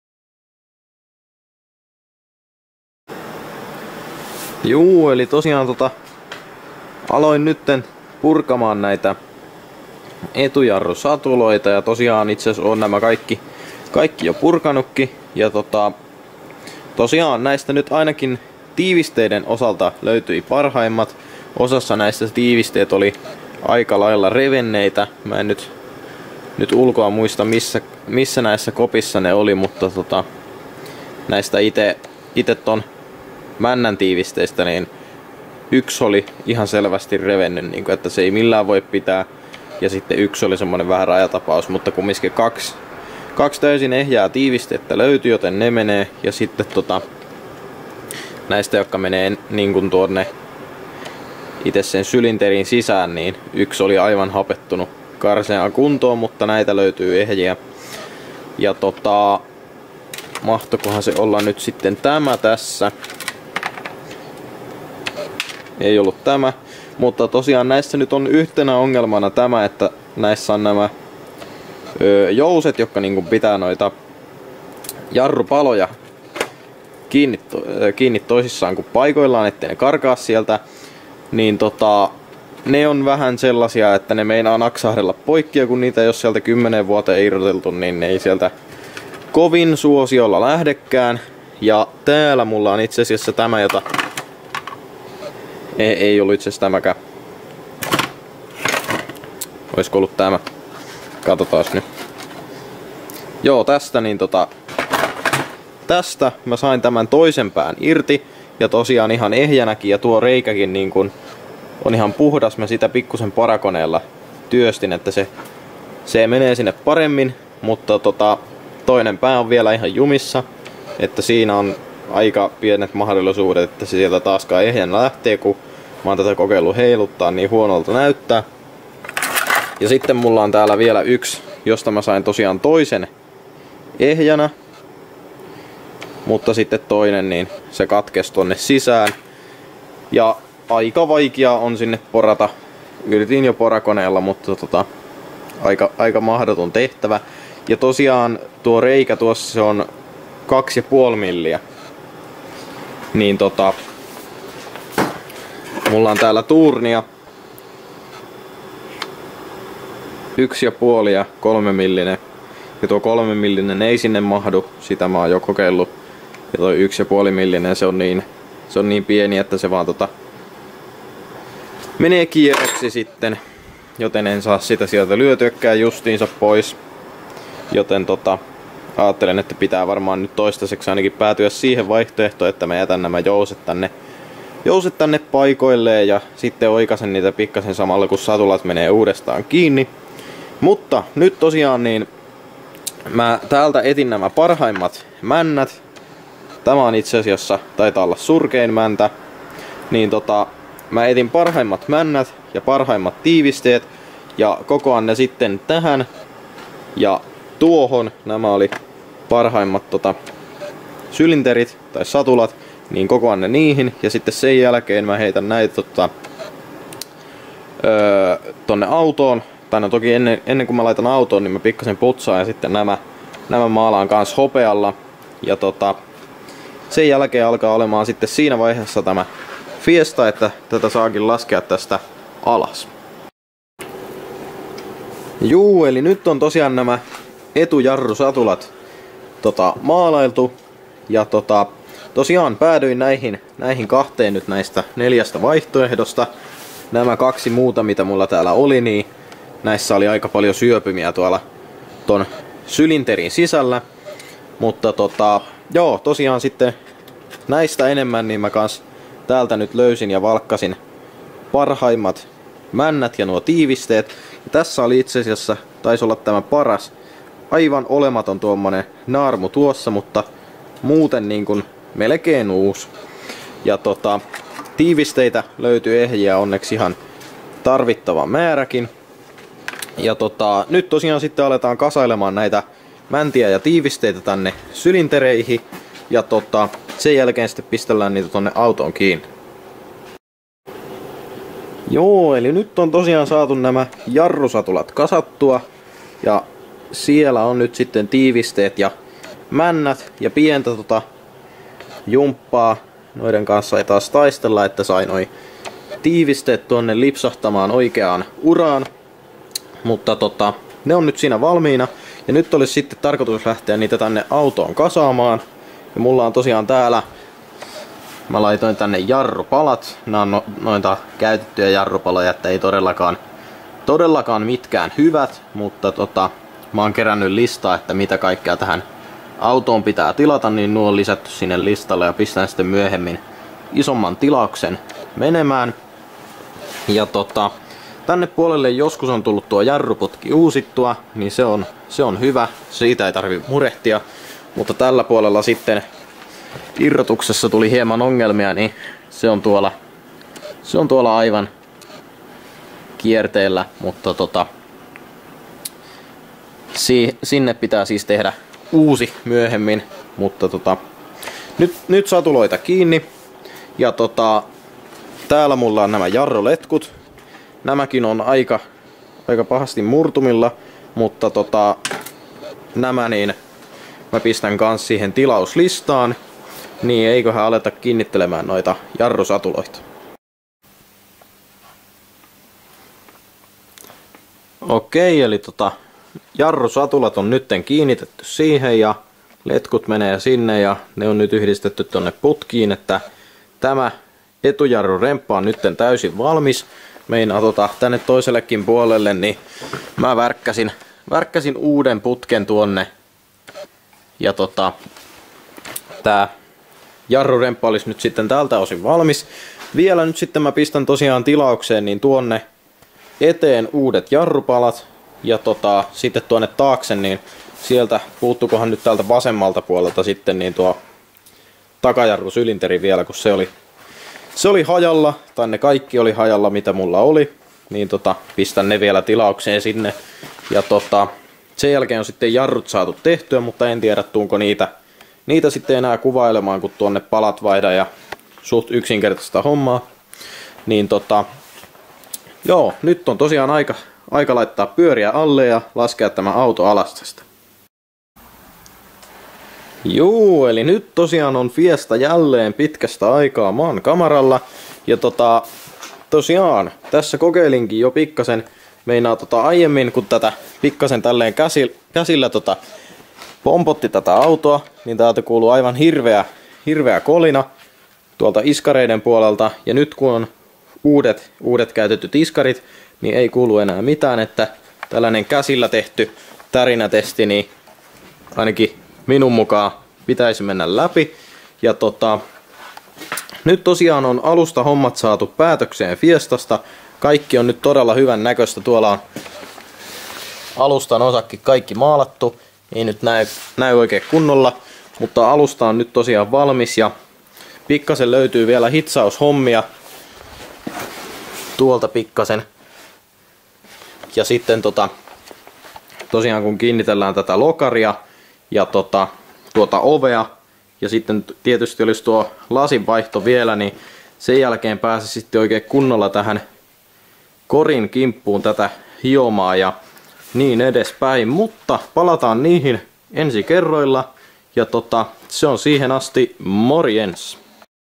Ford Fiesta MK3 Entisöinti Jakso 11: Etujarrut kasaan
Lyhyt projektivideo taas Fiestan edistymisestä, tällä kertaa saatiin etujarrut jo kutakuinkin valmiiksi.
Kerro kommentteihin mielipiteesi tai mietteesi videosta tai sen sisällöstä, sekä muista tykätä videosta 👍
Ja muistahan toki myös laittaa kanava tilaukseen mikäli sisältö on mieleistä.

Ford Fiesta MK3 1.4CLX Entisöintiprojekti